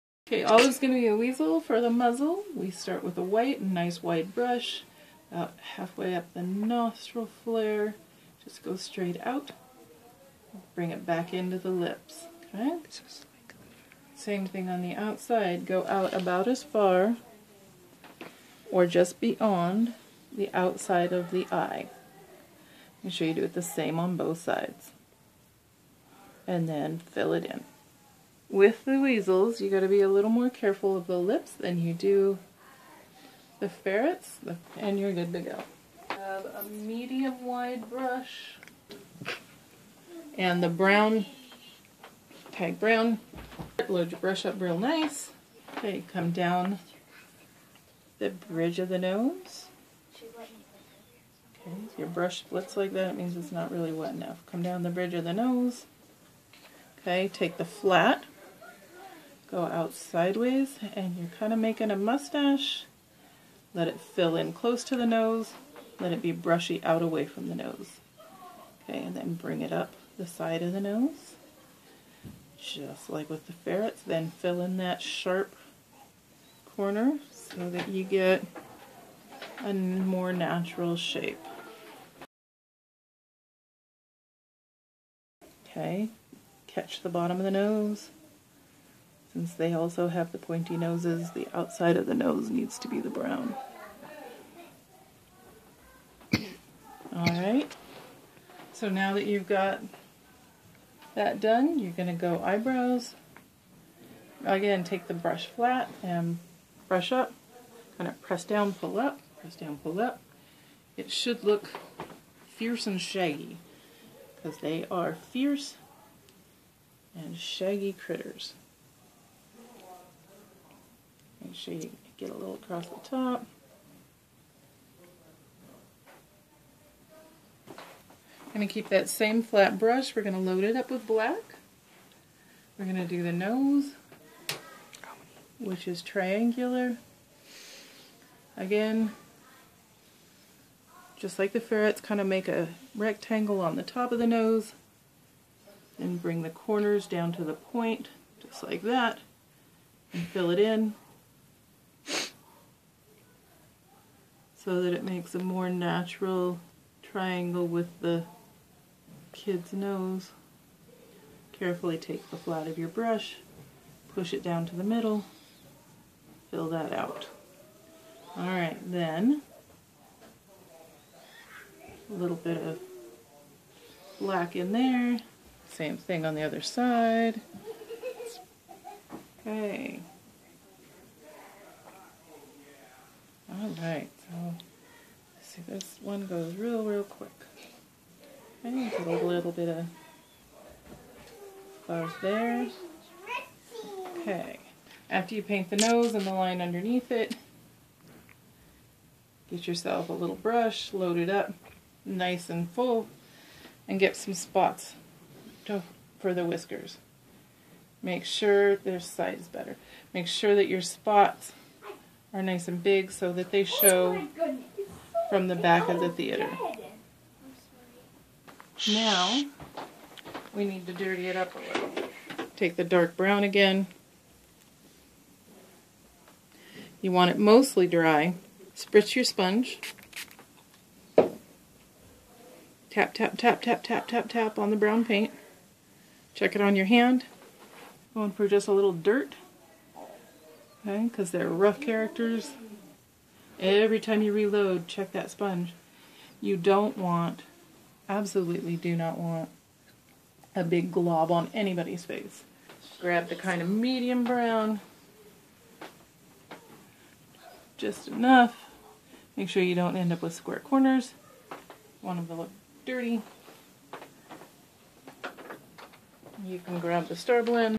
Wind in the Willows Weasel Face Paint
A face paint tutorial for the quick Weasel mask used for the Merc Playhouse Tom Zbyszewski's Children's Theater production of Wind in the Willows, May 2016. The Merc is located in Twisp, WA.

The production values aren't high, but I hope this will be helpful.

Products used:
Wolfe White
Wolfe Black
Tag Brown
Tag Earth
StarBlend Ebony

For products NOT available through Face Paint Forum Shop, I've added URLs for the products I've used ... hopefully they'll stay current. I find Face Paint Forum Shop (at the time of uploading, anyhow) tends to have the best prices, though there are many other lovely shops who'll be happy to help you out.